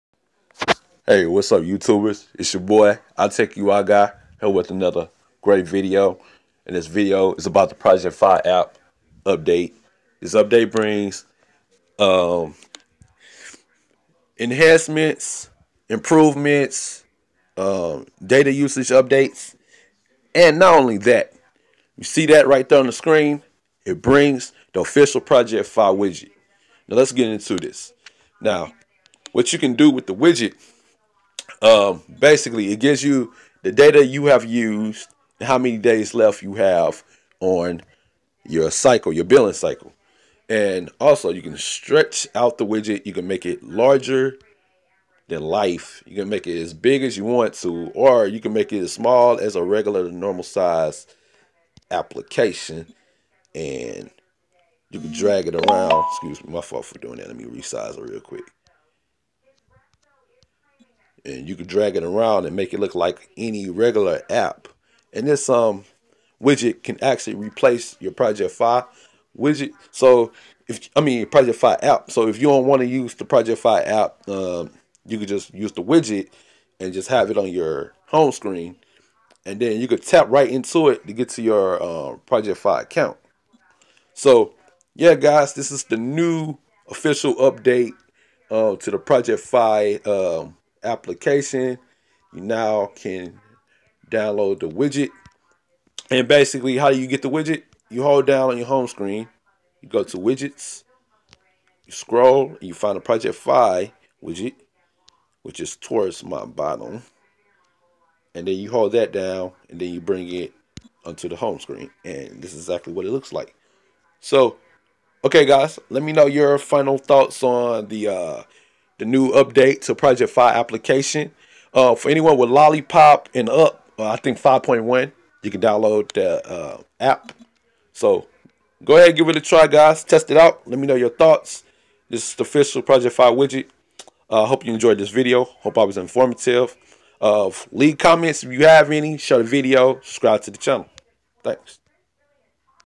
Google Project Fi App Update- Widget
Hello everyone. If you are on Google's Project Fi, the Project Fi app update now brings a widget.  The wait is finally over! See, with this widget, you can keep track of the data you have used and the days you left on your billing cycle. There are indicators on the widget to alert you when you are close to your data limit. You can also resize the Project Fi Widget to fit on screen how you want it.